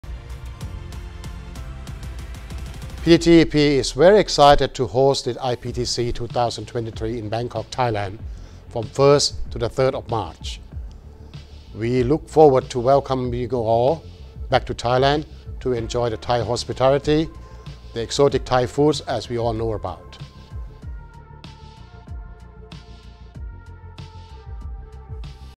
Montri Rawanchaikul - PTTEP as Host Organisation for IPTC 2023
IPTC 2023 Executive Committee Chair and CEO, PTT Exploration and Production Public Company Limited, Montri Rawanchaikul introduces PTT Exploration and Production Public Company Limited (PTTEP) as the Host Organisation for IPTC 2023.